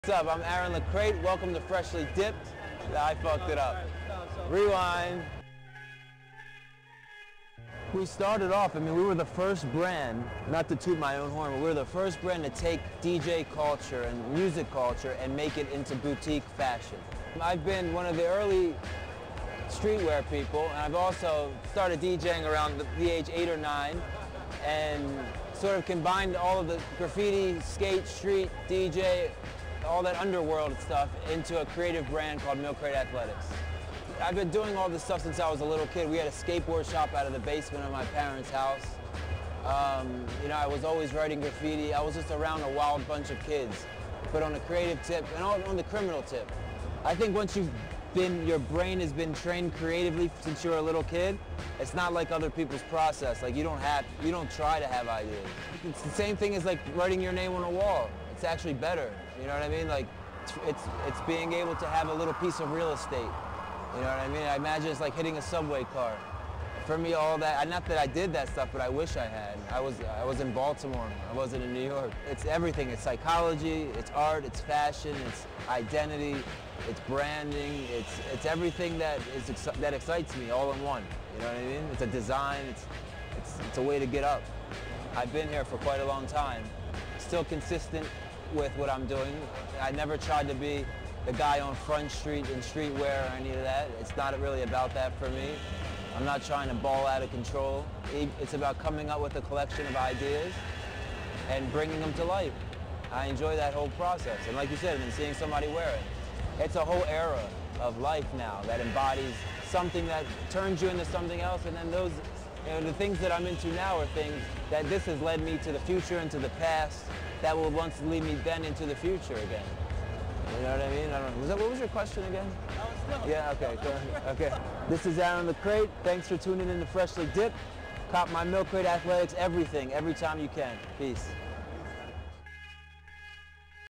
Freshly Dipped: Milkcrate & Aaron LaCrate drop science
The world famous Black Eyed Peas website feature acclaimed design and producer Aaron LaCrate of MILKCRATE ATHLETICS, speaks about  being 100% legit.